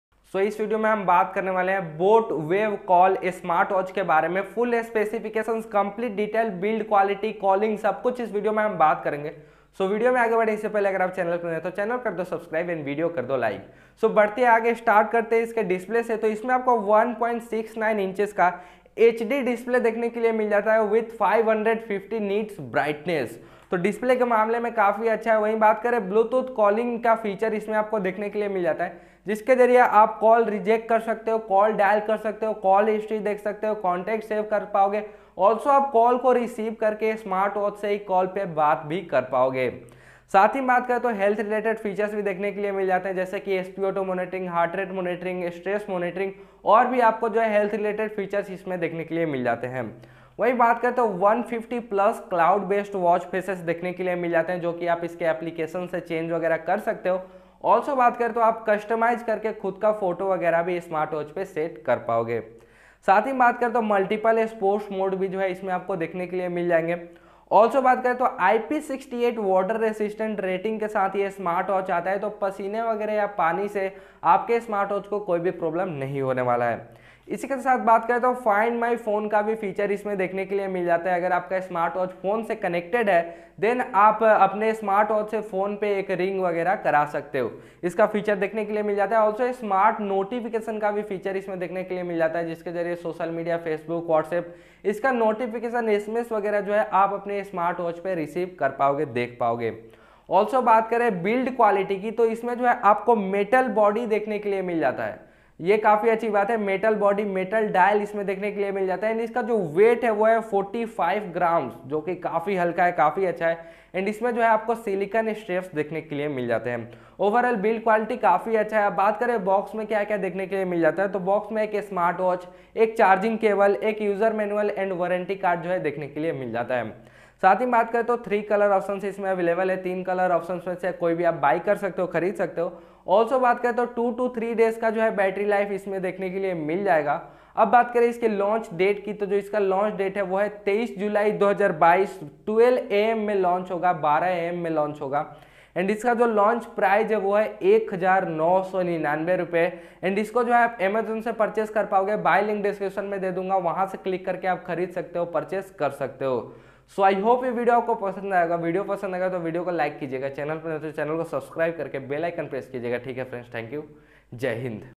Boat Wave Call Smartwatch - 1999, Calling, Build Quality, | Full Specifications and Details
In This Video I Will Tell You About Boat Wave Call Bluetooth Calling SmartWatch Full Specifications and Details

Buy Link 🔗
Boat Wave Call